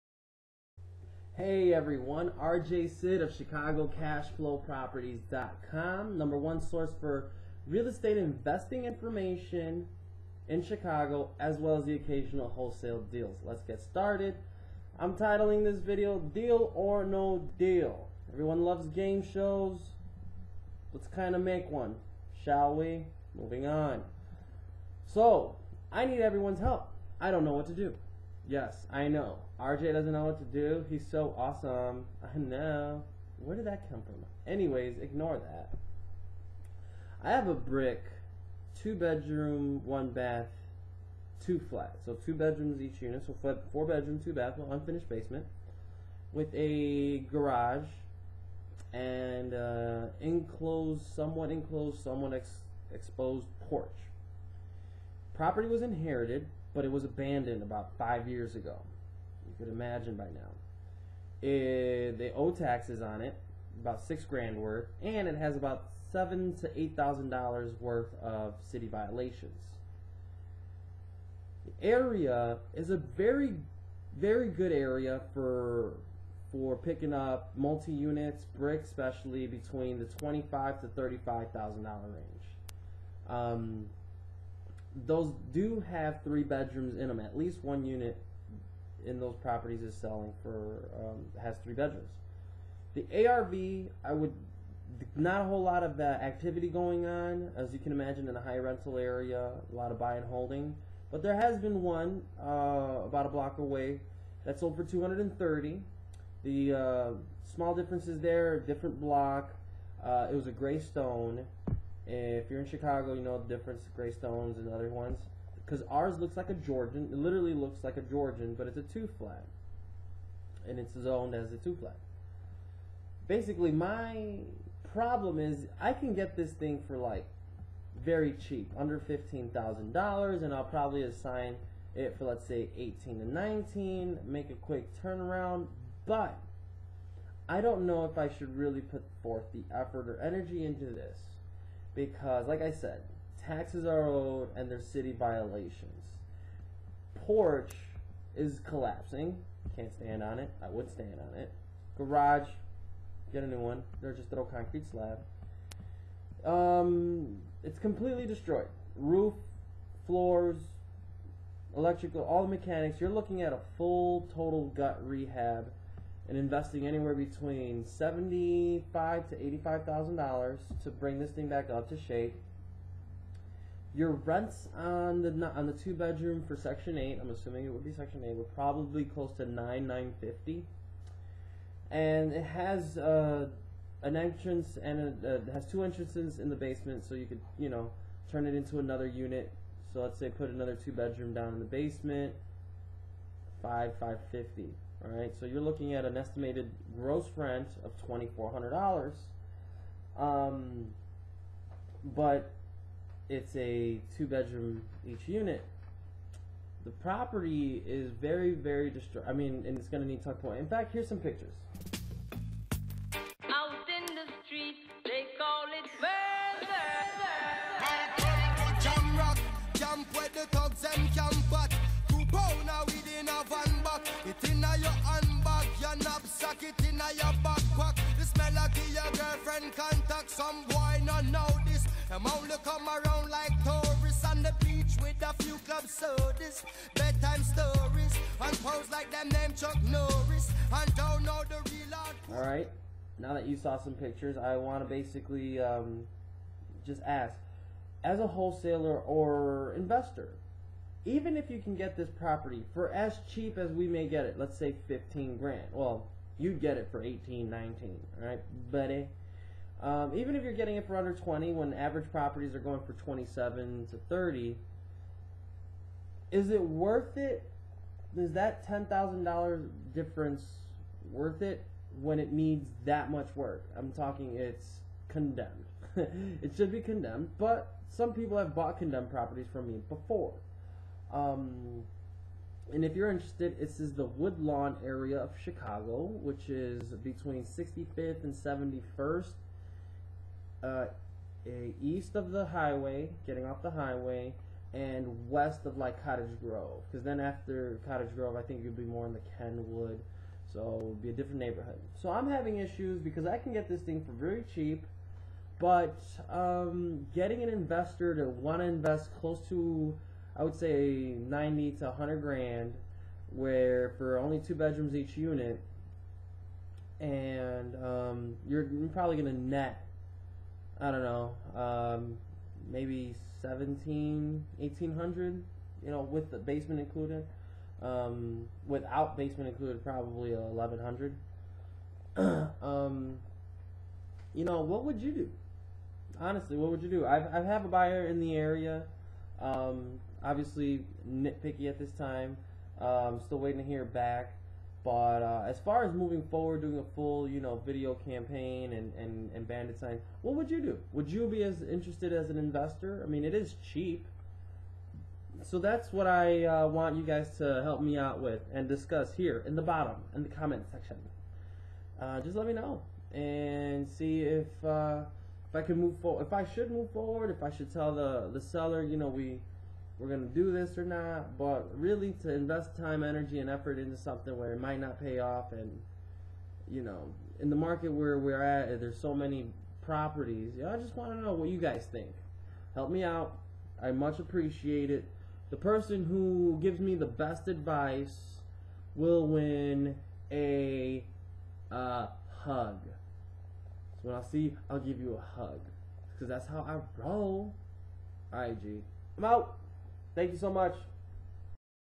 Chicago Real Estate Property: Deal or no Deal?
HEY everyone! RJ Cid here of ChicagoCashFlowproperties.com

I want YOUR help regarding a current dilema I have with a property. It looks like a good wholesale property for an investor, but maybe the damage is too much and not enough return? Let me know what you think!

estate investment real
real estate investor
investors real estate
investor in real estate
homes in chicago
estate investing